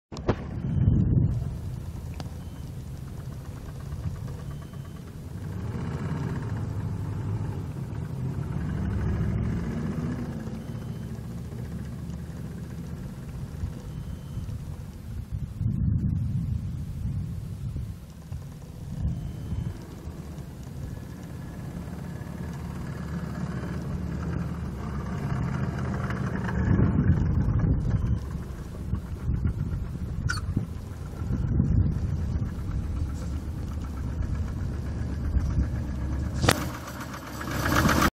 1997 GMC Sonoma Ext. Cab pickup truck for sale at auction | bidding closes June 18, 2019
1997 GMC Sonoma Ext. Cab pickup truck
108,694 miles on odometer
2.2L L4 OHV 8V gas engine
Cruise
205/75R15 tires
Engine knock
1GTCS1946VK508827
It's located in Kadoka, SD, and we're selling it to the highest bidder regardless of price.